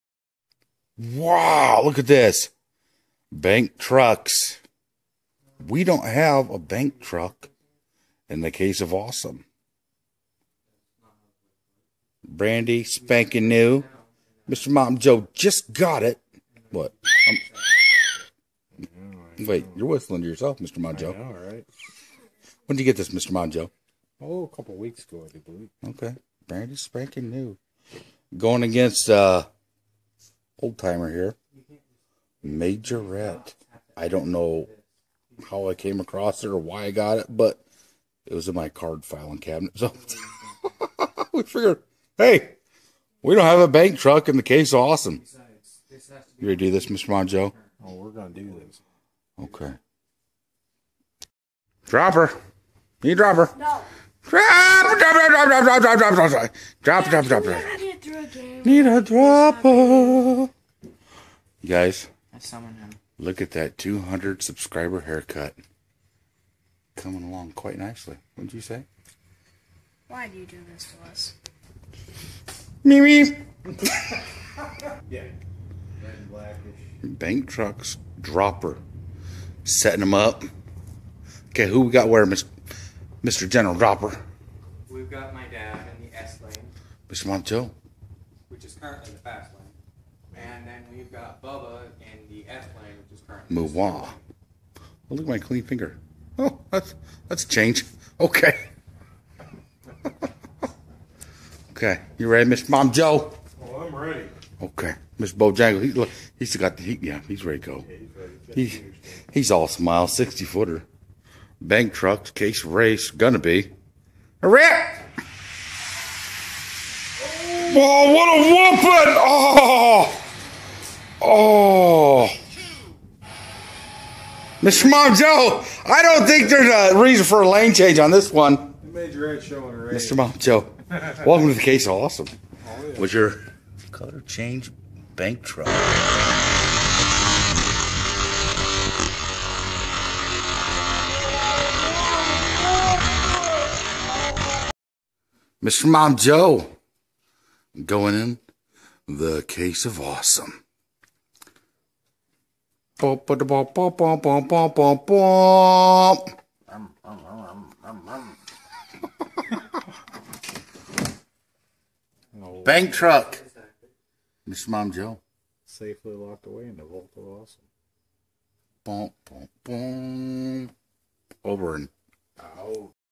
Majorette vs Hot Wheels... Bank/Box Truck Race
MrMomsRacing 1/64 diecast DHR downhill drag racing on four 60 ft lanes. Majorette vs Hot Wheels... Bank/Box Truck Race